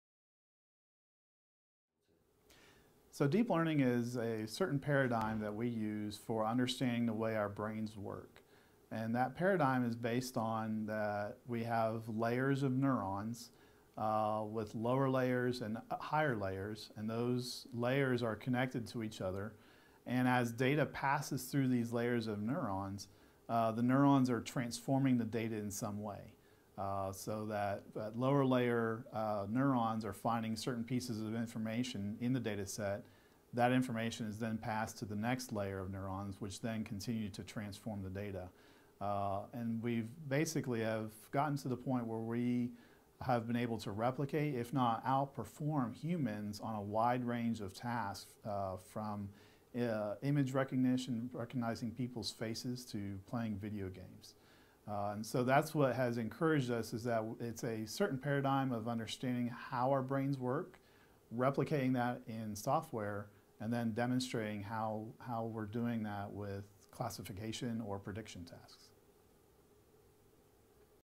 How would you define Deep Learning? - Robert Patton
Video recorded at the Workshop On mining Scientific Publications, 19th-23rd June at The University of Toronto, as a part of JCDL 2017 (Joint Conference on Digital Libraries).